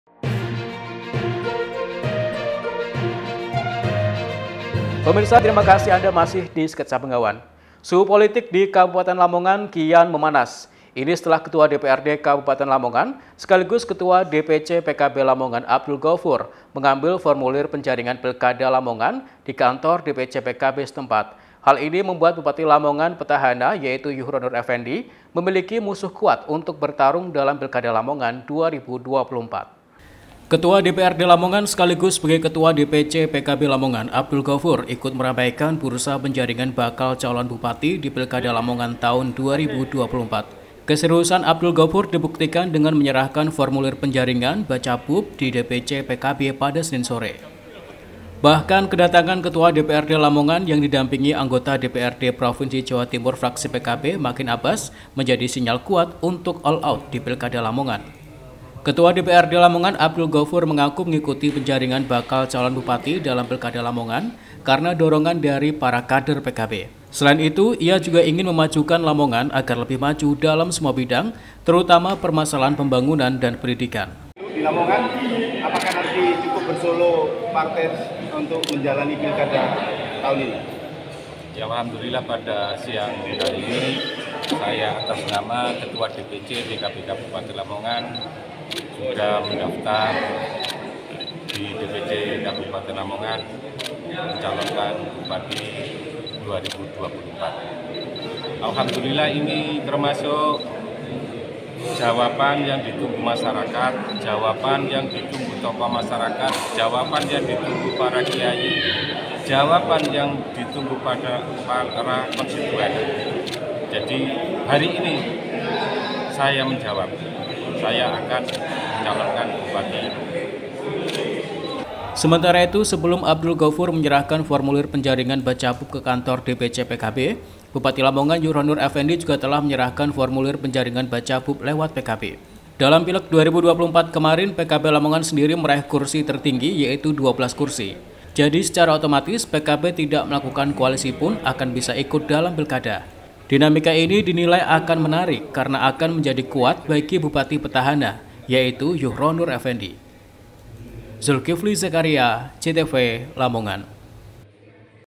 Ketua DPRD Siap Bertarung Dengan Bupati di Pilkada Lamongan
SKETSA BENGAWAN - Suhu politik di Kabupaten Lamongan kian memanas. Ini setelah Ketua DPRD Kabupaten Lamongan sekaligus Ketua DPC PKB Lamongan Abdul Ghofur, mengambil formulir penjaringan Pilkada Lamongan di kantor dpc pkb setempat. Hal ini membuat Bupati Lamongan Petahana Yuhronur Efendi, memiliki musuh kuat untuk bertarung dalam Pilkada Lamongan 2024.